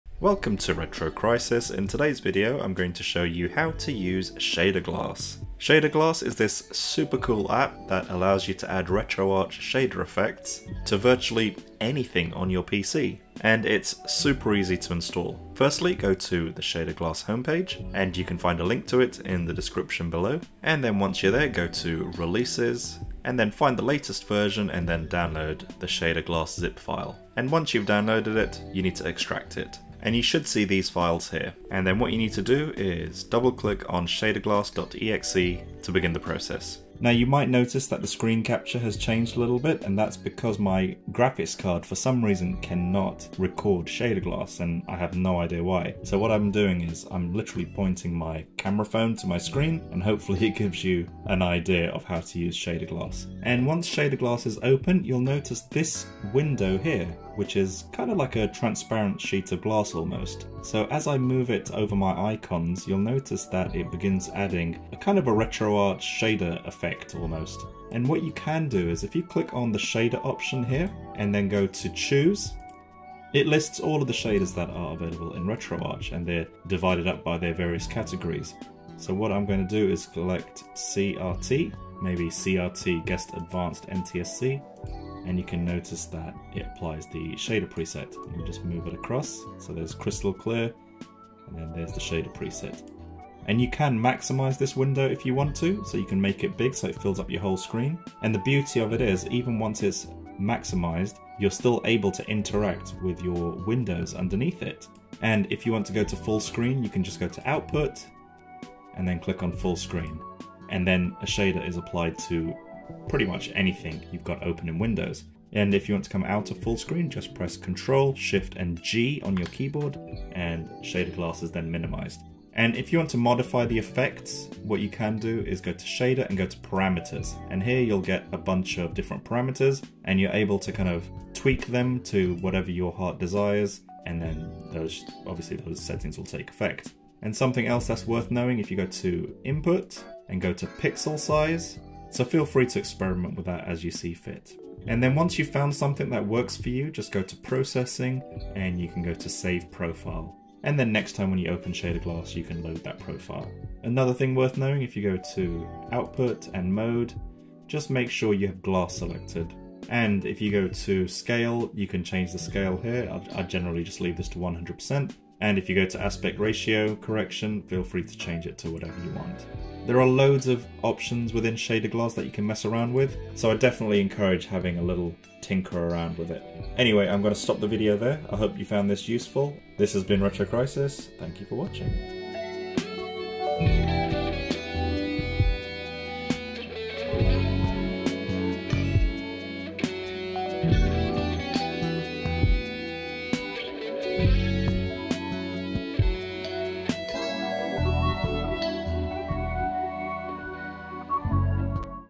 Add RetroArch Shaders to EVERYTHING - ShaderGlass guide Shader Glass
ShaderGlassOverlay applying retro shaders on top of Windows desktopFeatures:- applies shader effects on top of any window on the desktop in realtime- includes 300+ shaders from RetroArch library covering:- - CRT monitor simulation- - image upscaling- - TV / VHS simulation- - softening, denoising, blur and sharpen - works with most emulators and retro platforms (in windowed/borderless modes) including DOSBox FS-UAE Altirra ScummVM AGS any many more- you can even use it on top of YouTube or Twitch, albeit with some option tuning necessary to clean up video scaling- works with OBS (in Game Capture mode)

0:00 - Introduction to ShaderGlass
0:15 - Download ShaderGlass
0:30 - Open ShaderGlass
0:36 - Sorry
0:53 - What is ShaderGlass?
1:08 - Selecting Shaders
1:33 - Fullscreen ShaderGlass
2:02 - Modify Shaders
2:17 - Other Options
2:25 - Saving ShaderGlass Profile
2:33 - More ShaderGlass Options